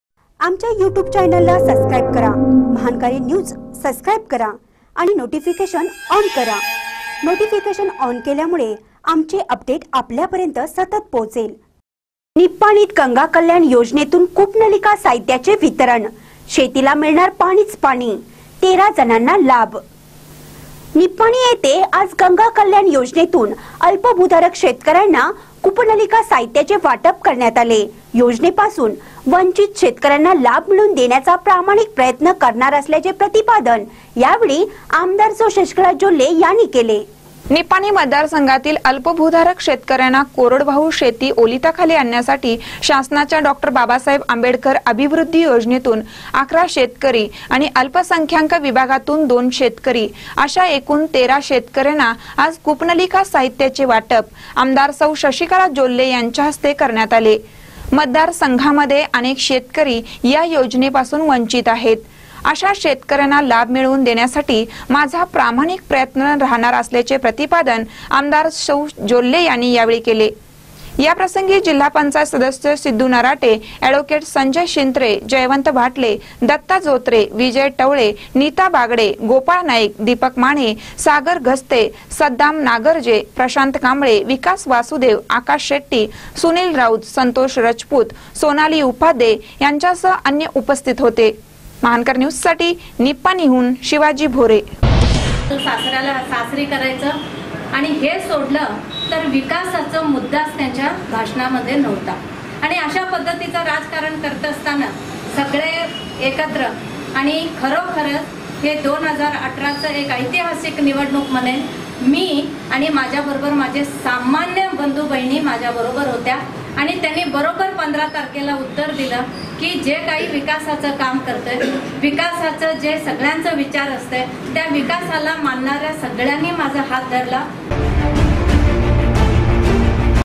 निपाणीत गंगा कल्याण योजनेतून कूपनलिका साहित्याचे वितरण
निपाणीत गंगा कल्याण योजनेतून कूपनलिका साहित्याचे वितरण : शेतीला मिळणार पाणीच पाणी, १३ जणांना लाभ